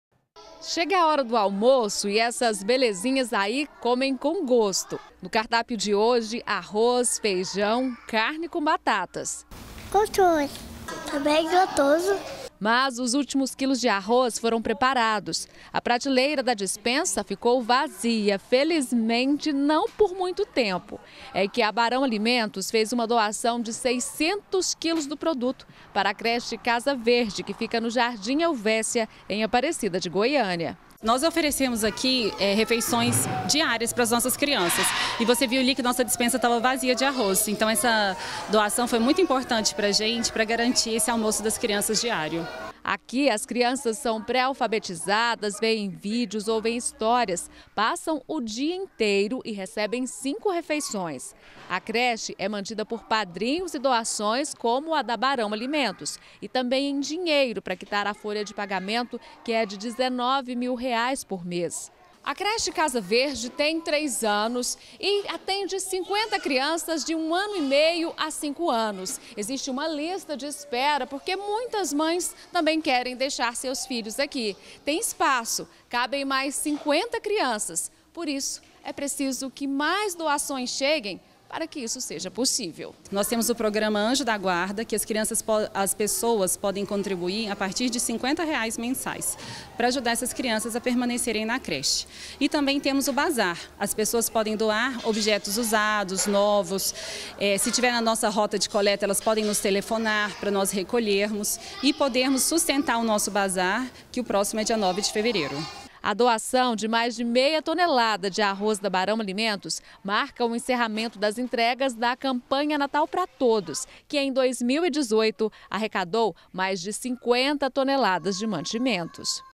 NATAL PRA TODOS: 600 KILOS DE ARROZ ENTREGUES EM CRECHE
A campanha natal para todos já acabou, mas as doações ainda estão chegando para as entidades beneficiadas. Você vai ver agora que mais de meia tonelada de arroz foram entregues pela barão alimentos para a creche casa verde em Aparecida.